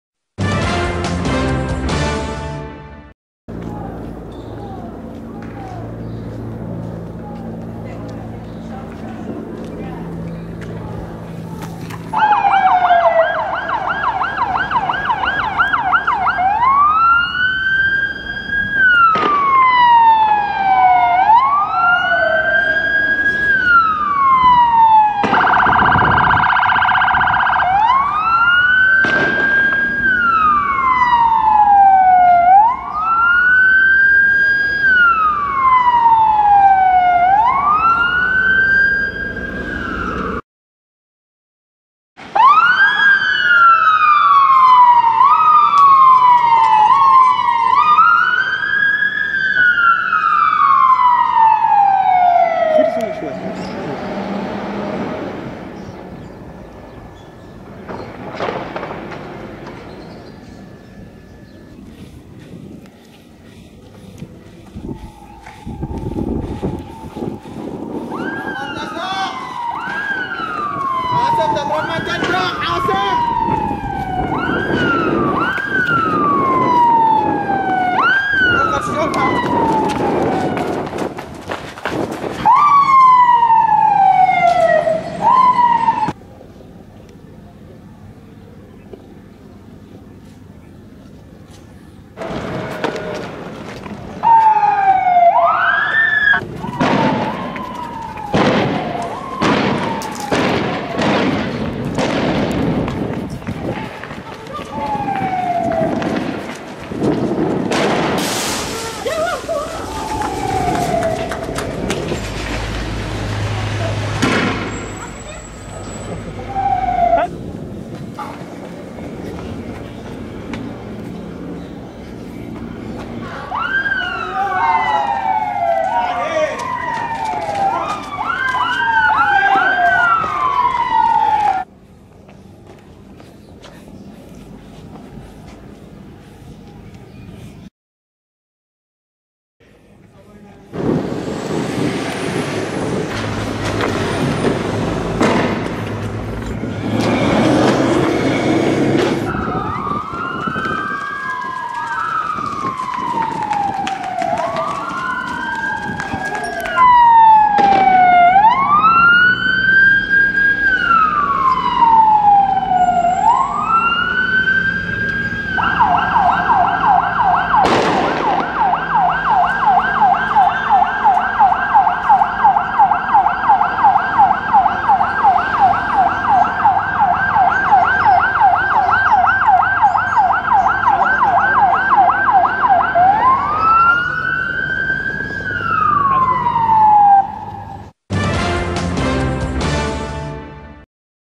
مواجهات براكين العزة في النويدرات تزامنا مع اليوم الختامي لسباق الفورملا 1 Bahrain
شهدت قرية النويدرات مساء أمس الأحد (6 ابريل/نيسان 2014) إشتباكات ومصادمات بين محتجين ومرتزقة النظام الخليفي، شرارة الصدامات بدأت بعد نزول حشود من المتظاهرين للشوارع وتوجههم نحو نقطة لتمركز القوات مرددين شعارات مناهضة للنظام، دقائق معدودة وبدأ مسلسل القمع من قبل النظام وهو ما حول المنطقة لساحة إشتباكات وصدامات استمرت حتى ساعات الليل الأولى.